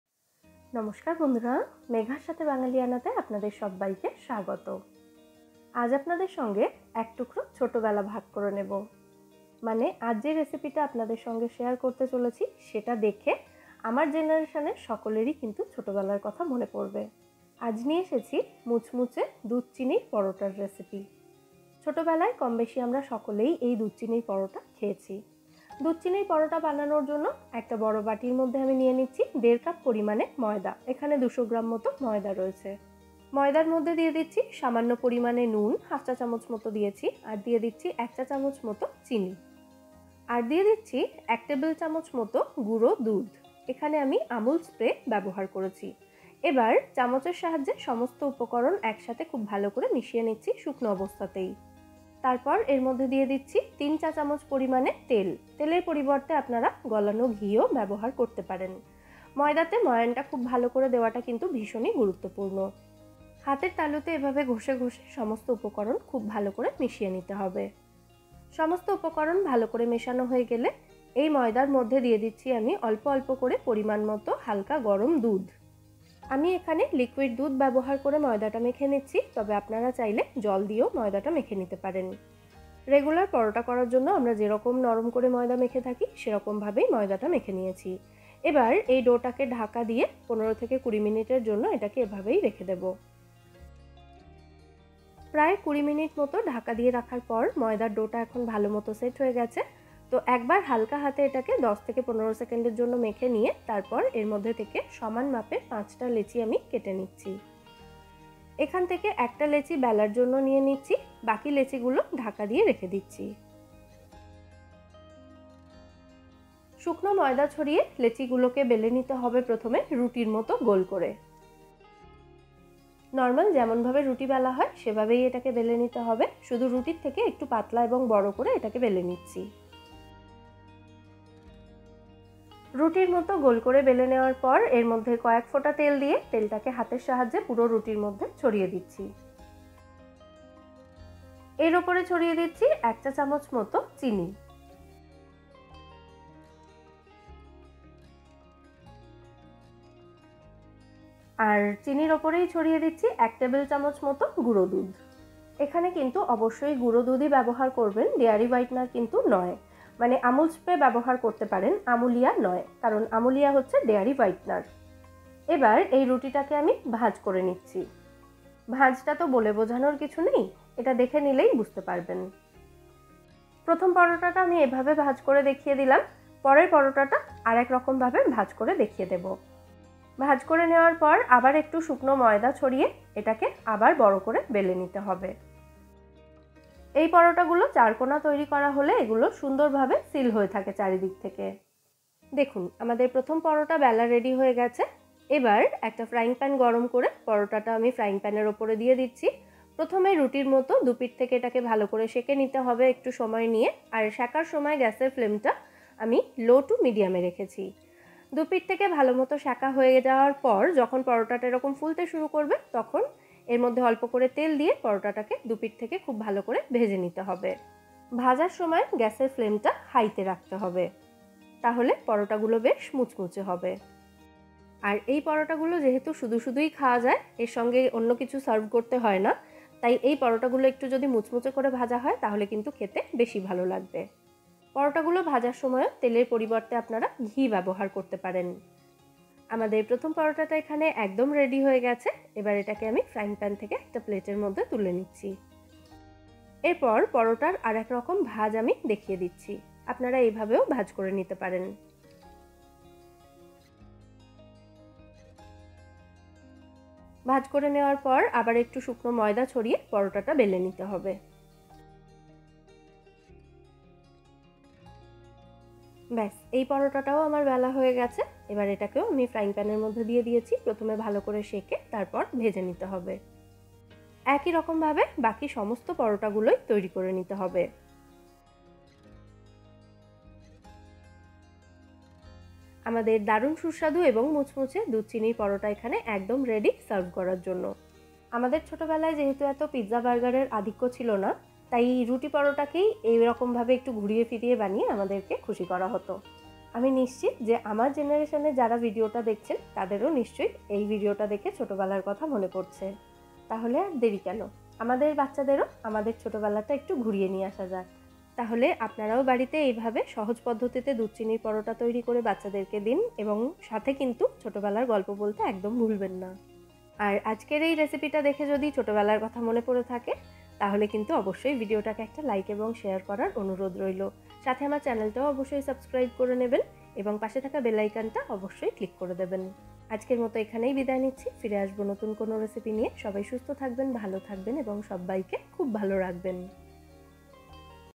দুধ, চিনির মুচমুচে পরোটা, এই রেসিপি ছোটবেলার কথা মনে করিয়ে দেবে😍 | dudh chinir muchmuche porota
Ingredients
1.5cup all purpose flour (maida)
Milk powder as required
Liquid milk (luke warm)
Oil or ghee (3tsp for dough & more to fry)